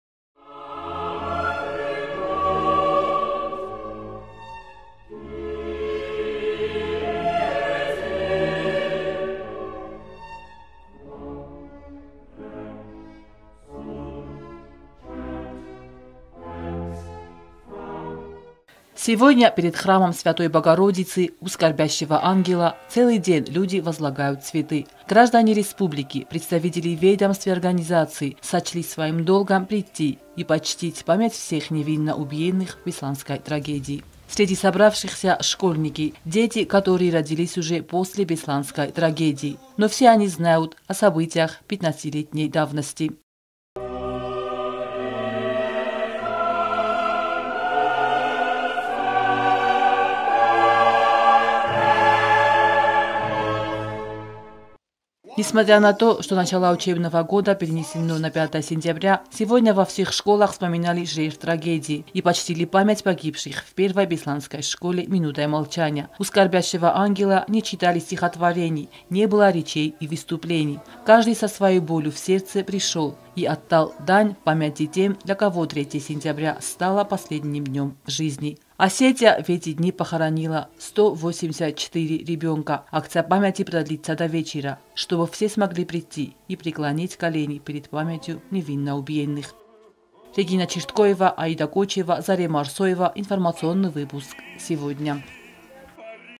Сегодня как в самом Беслане, так и в обеих частях Осетии проходят памятные мероприятия
Трагедия не оставила равнодушным ни одного здравомыслящего  человека во всем мире.  С терроризмом сталкивались практически во всех странах, но от такой зверской  жестокости в отношении невинных детей мир содрогнулся.  Эта дата навсегда останется самой скорбной в истории Осетии.